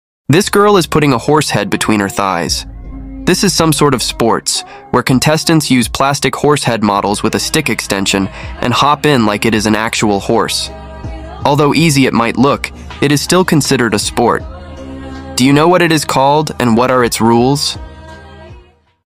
Horse doll riding! Hobby Horse jumping competition!
Cred: @karos.hobby.horse on IG

This girl is putting a horse head between her thighs. This is some sort of sports, where contestants use plastic horse head models  with a stick extension, and hop in like it is an actual horse. Although easy it might look, it is still considered a sport. Do you know what it is called, and what are its rules?